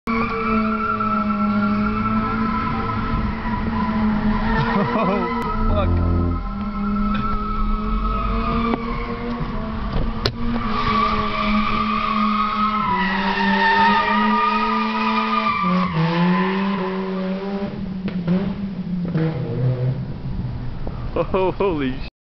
RX8 Burnouts Auckland, Rotary
Vedran doing burnouts in his RX8